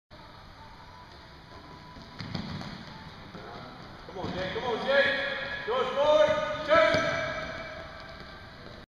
VSP MTP Jalen Hooks Court Agility
This is another agility and speed assessment at our annual Under Armour Basketball Combine. Killing it!